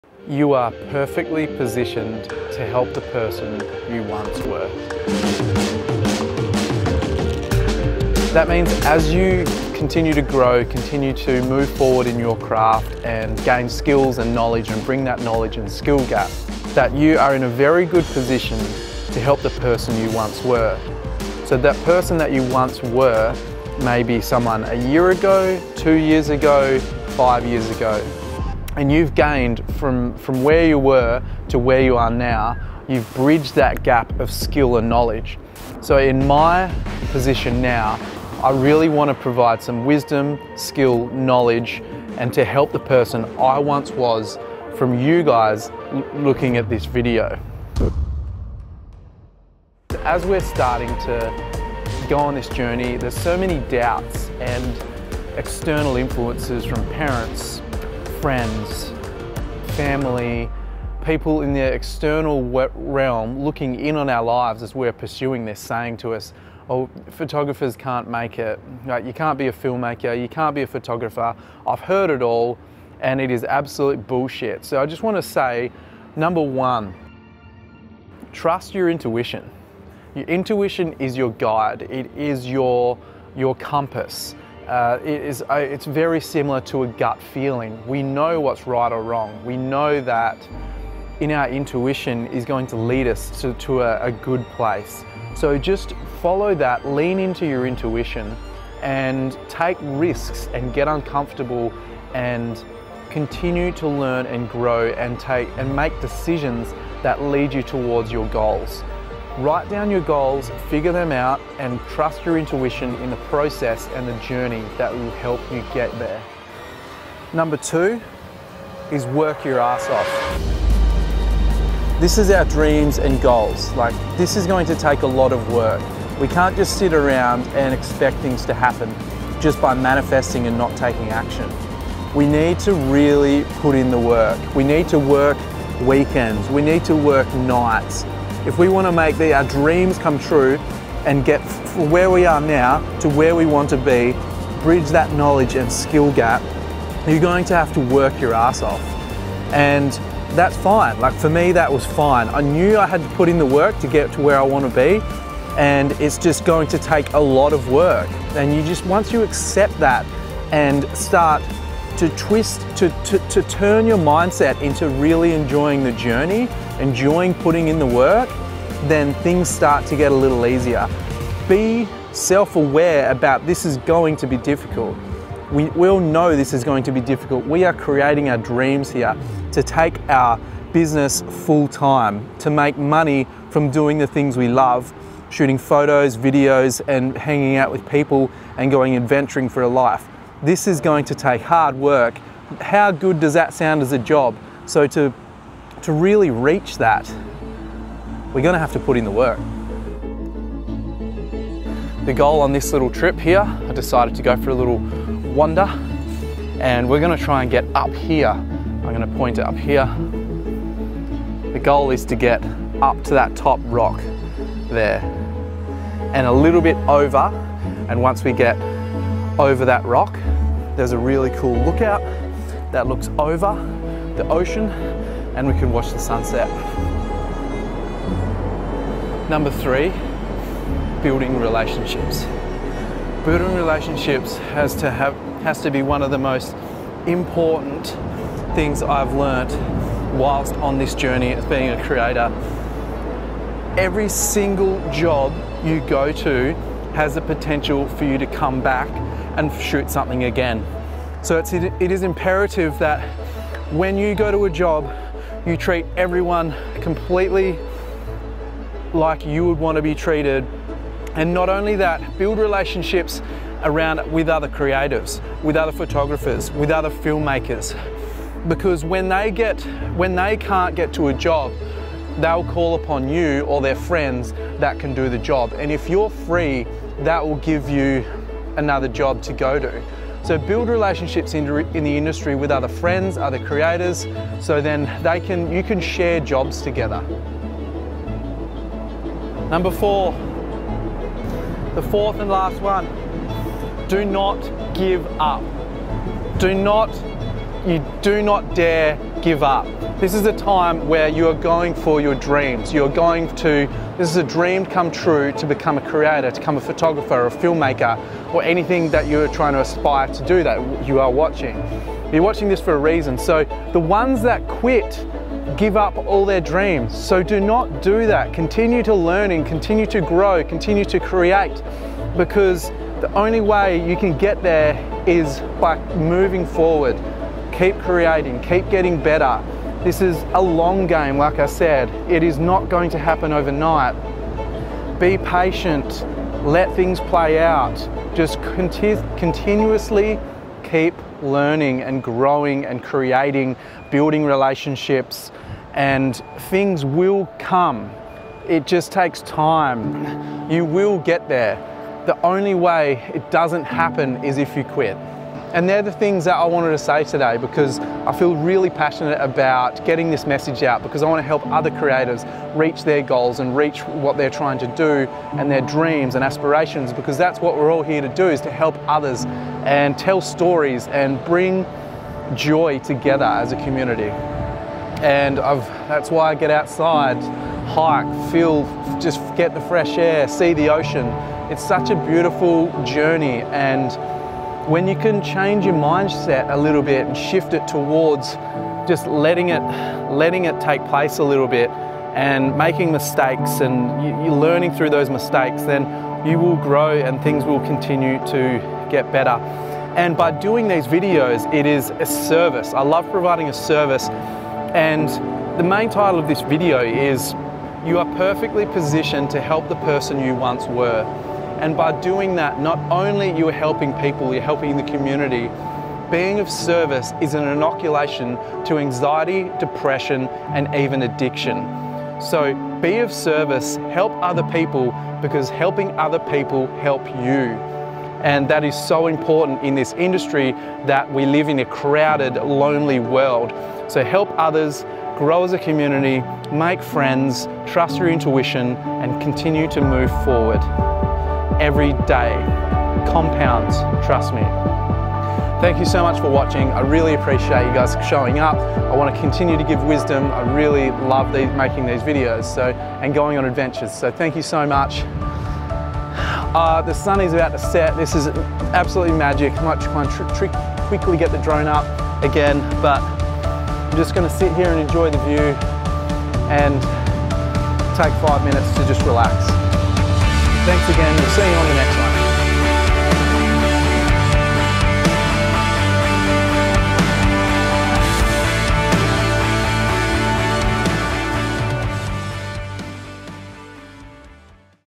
Help the person YOU once were!!
Helping the person you once were!. Being of service is an inoculation to anxiety, depression and even addiction. In this video I give my lesson learnt from my experiences in the industry to help you grow as a creative to reach your goals. I bring you into my past and what I wish I knew from the start of myself pursuing this as a career.  Enjoy